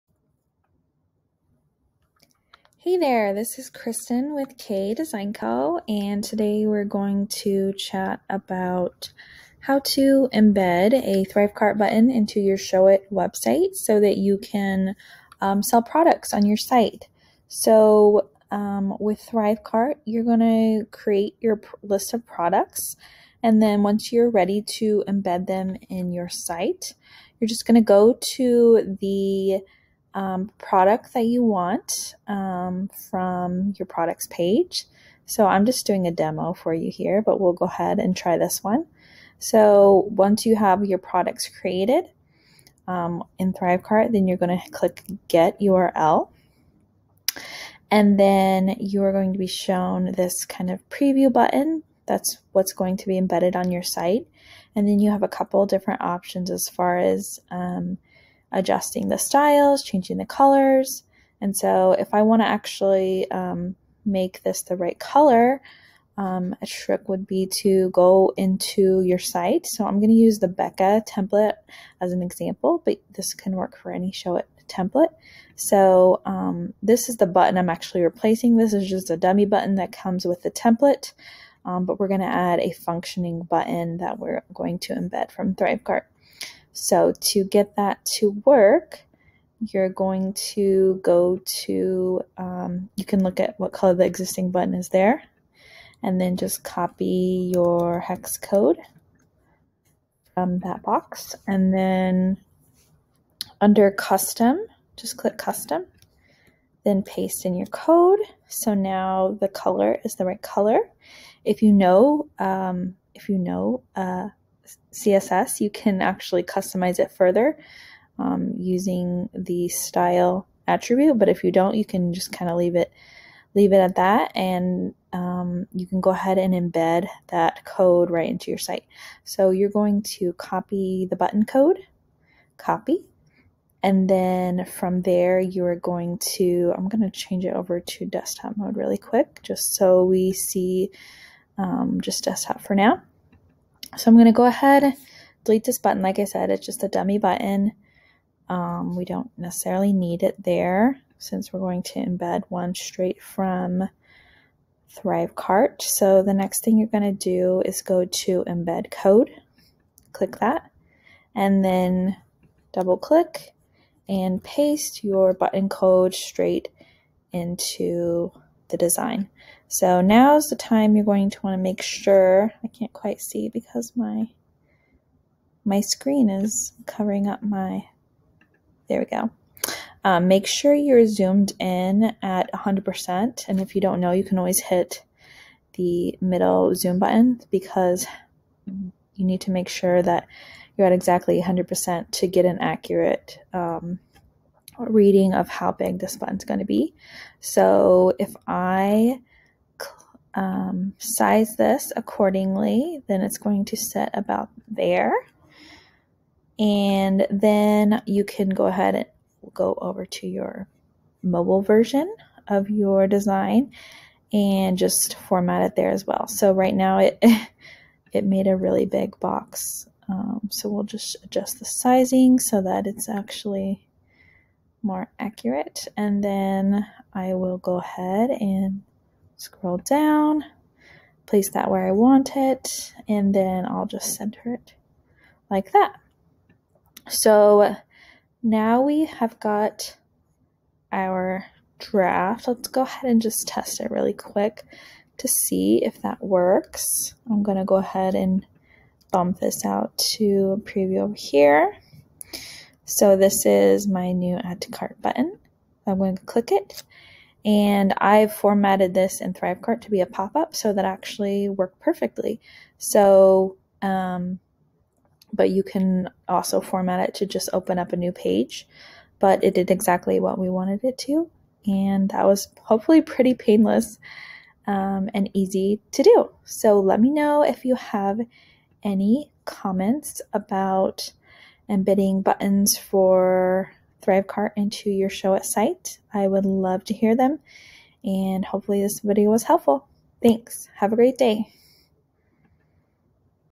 How to Embed a Thrivecart Buy Button into Your Showit Website
Ready to add e-commerce to your Showit site? It's easier than you think! Here's a quick and easy tutorial that shows you step-by-step how to add Thrivecart buy button into your Showit website including customizing your buy button's color using your template's built-in hex code color palette.

Don't forget to check out my Showit template shop to find the perfect website template for your coaching, photography, virtual assistant or social media manager biz